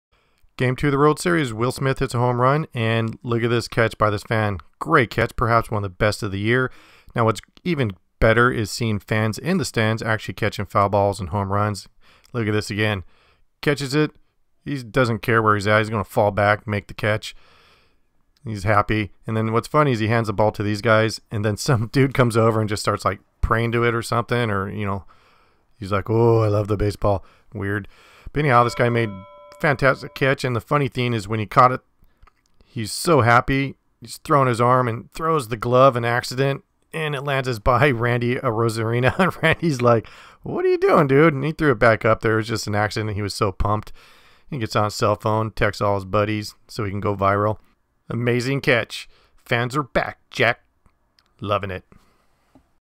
Top Fan catch of the World Series 2020
** This guy made perhaps one of the best catches of 2020. Granted we've had no fans until post season, but never the less still an amazing grab.

But the fan was so pumped while celebrating he tossed his glove onto the field.

Its been a long time since we saw fans catching foul balls and HR's in MLB.

** Playlist = World Series/Playoffs, Tampa Bay Rays and Los Angeles Dodgers playlist